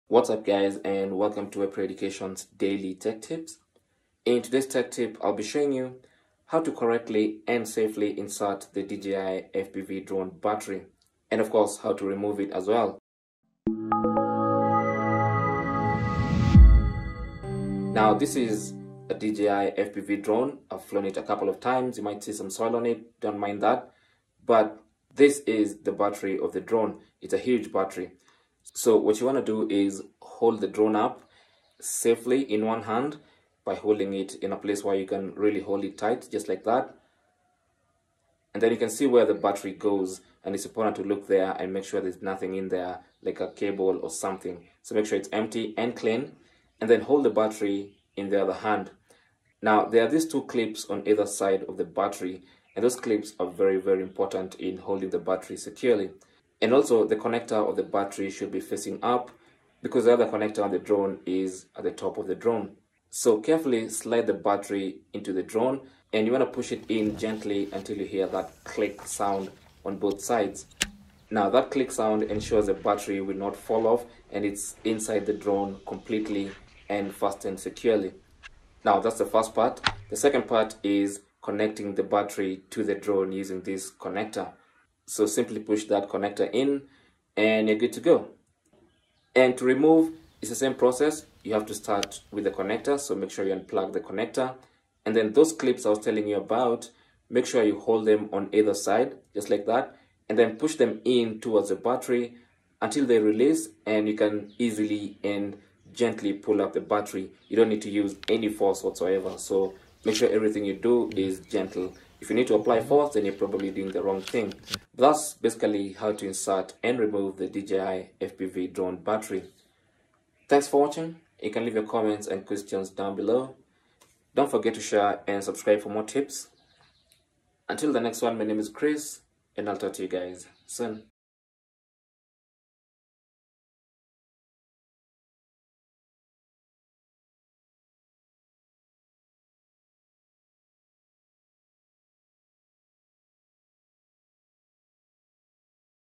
DJI FPV - How To Properly Insert and Remove Drone Battery
Learn how to insert and remove the DJI FPV drone battery. It's a simple process that requires gentle hands.

0:00 - Intro
0:28 - How To Insert the DJI FPV Drone Battery
1:27 - How To Remove Battery
1:55 - Outro